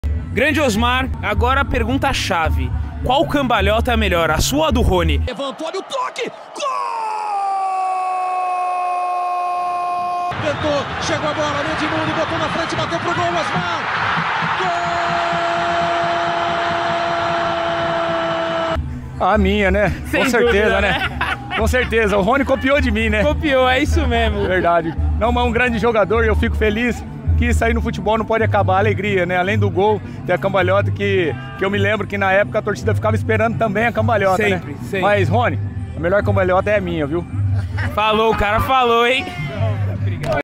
O RONY ME IMITOU NISSO AQUI - MELHOR CAMBALHOTA
Osmar brinca que o Rony imitou sua cambalhota

Faça seu REGISTRO na BETANO com o código promocional OSBOCCA e ganhe até 100% de bônus